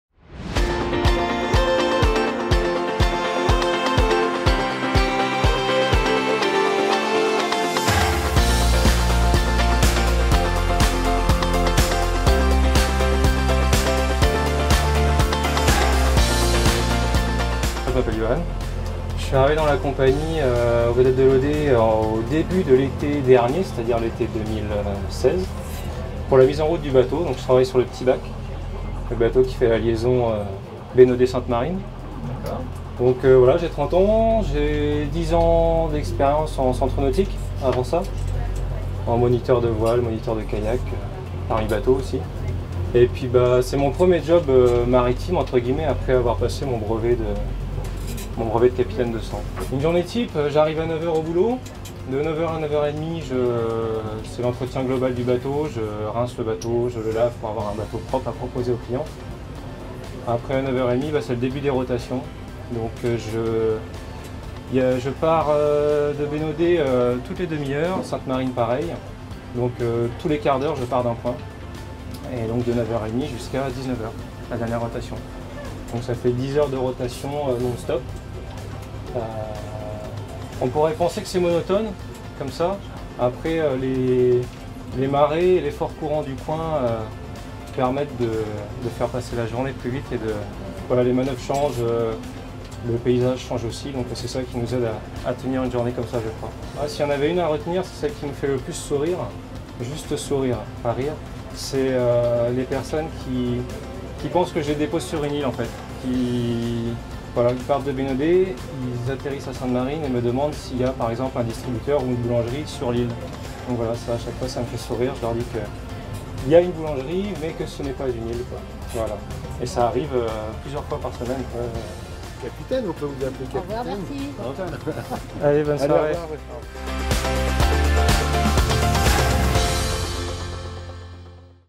Paroles de Marins - Yohann, capitaine du P'tit Bac
Découvrez à travers une série de reportages l’âme des Vedettes de l’Odet.
Ils sont Capitaine, Matelot, Mécanicien, Guide, Maître d’hôtel, Serveur ou encore Conseillère de vente… des métiers passionnants  présentés par des passionnés !

Act 1 : Yohann Ascoët – Capitaine du P’tit Bac